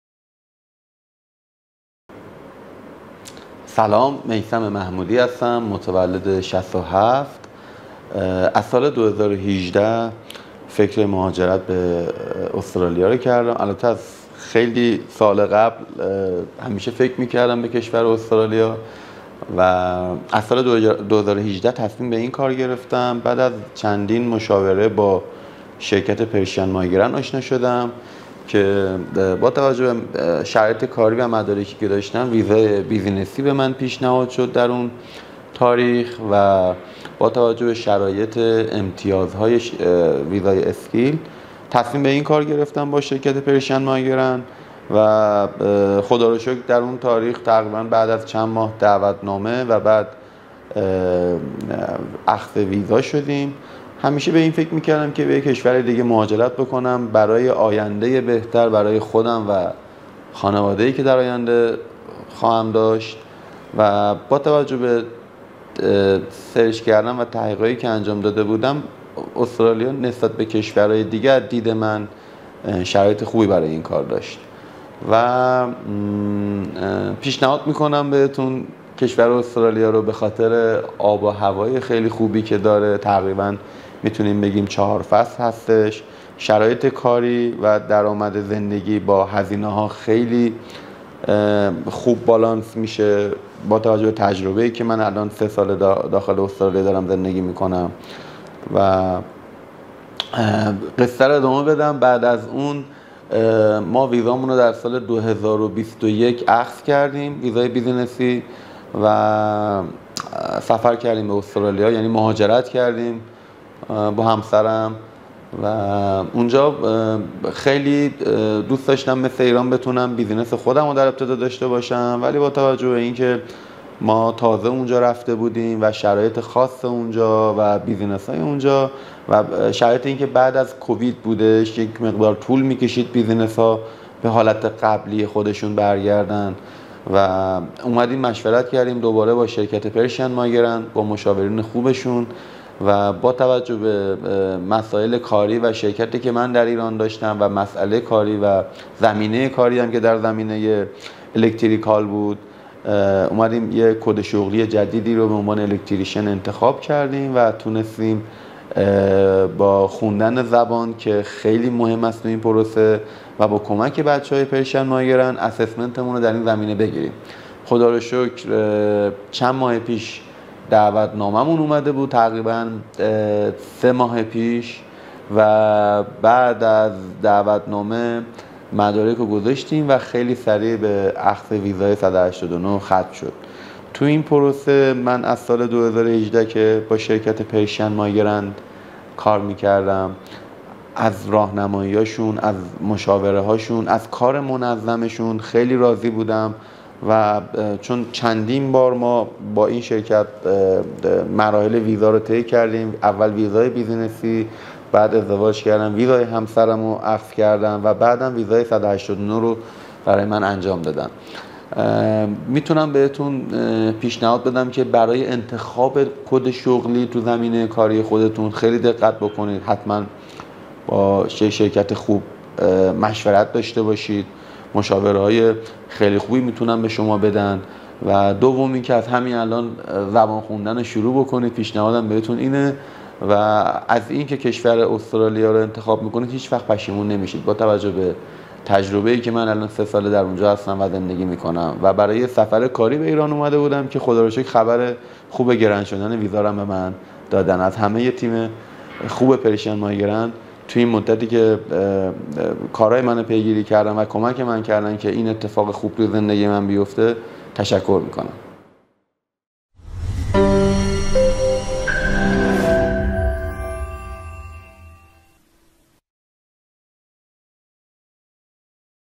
ویزای 188 و 189 استرالیا
🎆 تبریک به کلاینت عزیزمون آقای میثم محمودی برای دریافت ویزای 188 و ویزای 189 استرالیا

امیدوارم به زودی برای همه کلاینت های عزیز خوش خبر باشیم 👍

🔹اطلاعات تماس:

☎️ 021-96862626